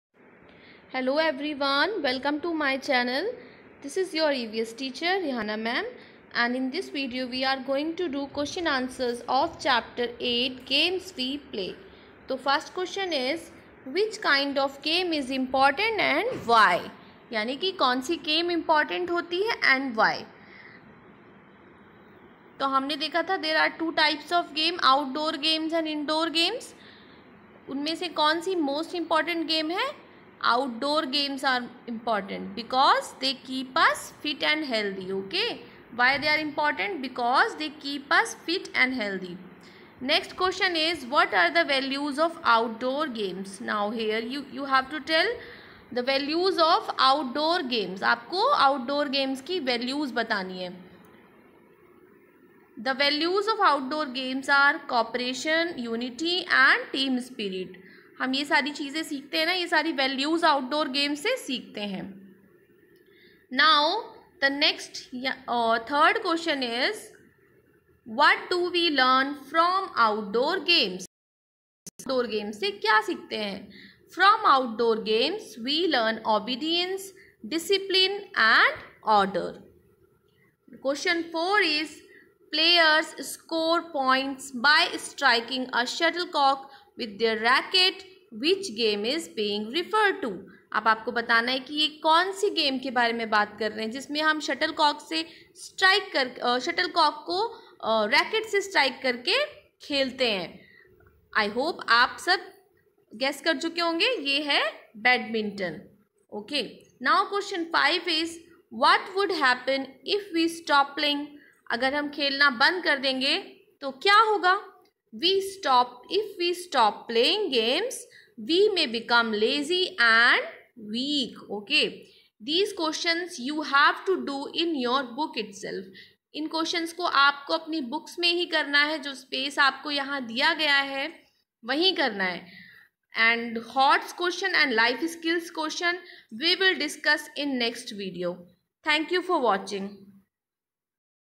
Games We Play, Q/A | Chapter-8, class 2 EVS
In this video I have discussed question answers of chapter 8 of class 2 EVS book Think Green.

CHAPTER 8
Games We Play
Question Answers